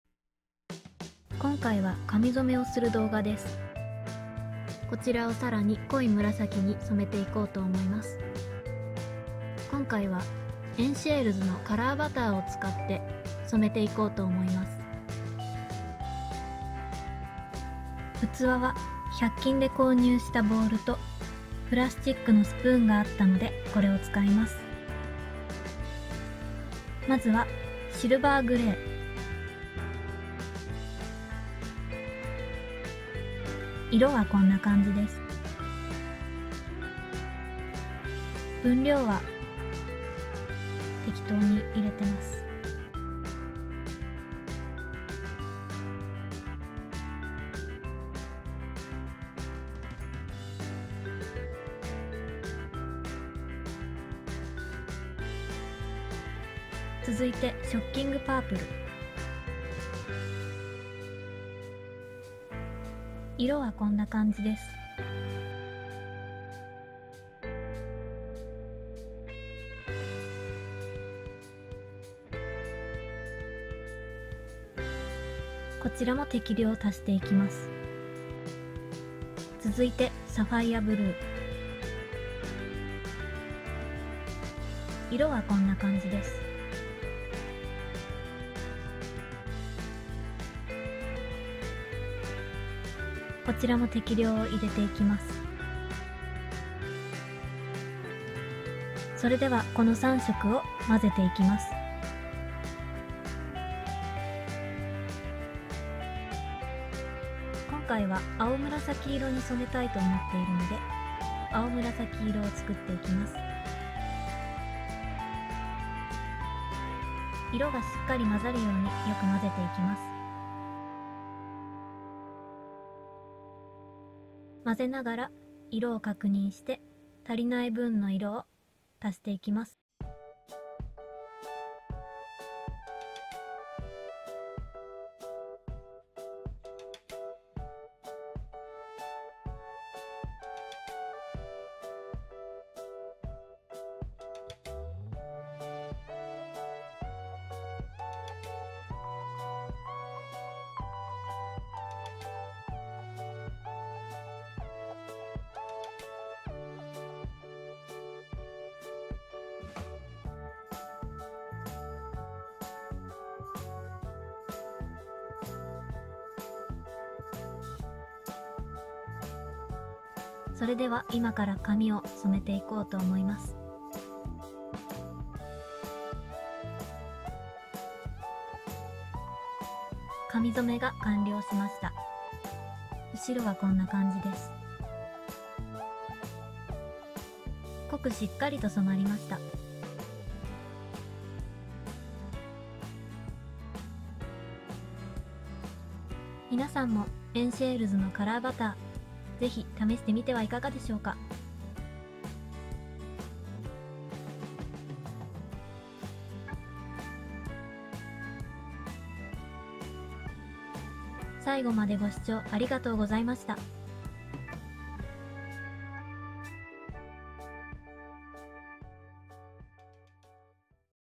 髪染め大作戦！カラーバターで青紫に染めてみた
今回は、3色のカラーバターを使って私の大好きな青紫色を作り、髪を染めてみました。
エンシェールズのカラーバターはとても発色がいい！（黒髪にはほぼ色が乗らないので、明るくした髪に使用するのをおススメします。）
色作りもまた楽しみのひとつ。